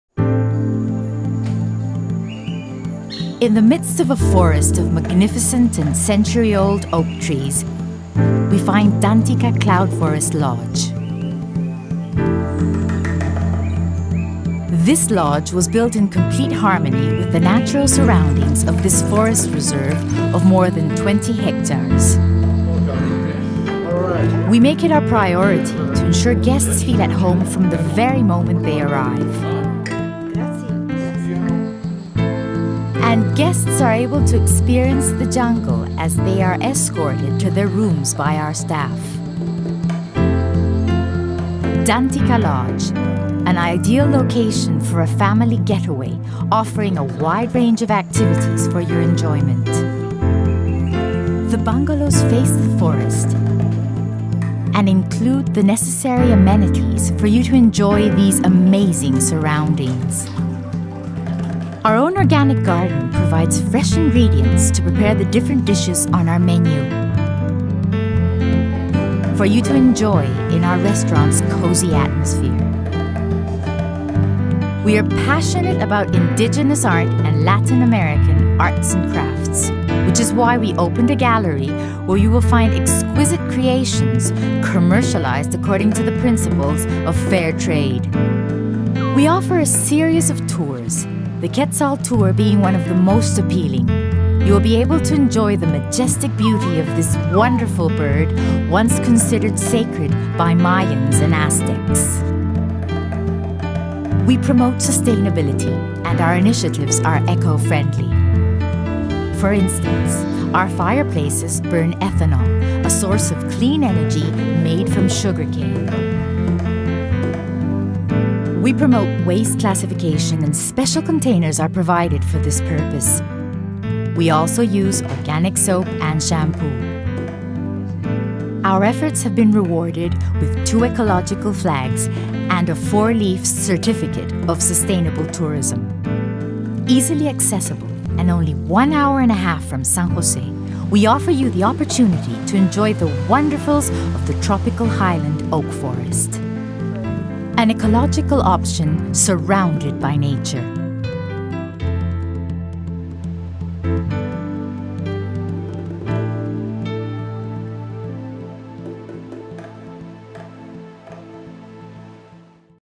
Dantica Cloud Forest Lodge
The Dantica Cloud Forest Lodge is situated high in the mountains of the Talamanca mountain range, at an altitude of 2. 600 meters above sea level, in the middle of the Upper Montane Cloud Forest.

Our eight luxurious bungalows are located in the heart of the Los Santos Forest Reserve, some 84 km from the capital San José towards Southern Costa Rica. Constructed on the top of a hill in the middle of this valley full of Quetzals, these bungalows offer a spectacular view of the cloud forest. The small village of San Gerardo de Dota is only a few kilometers away.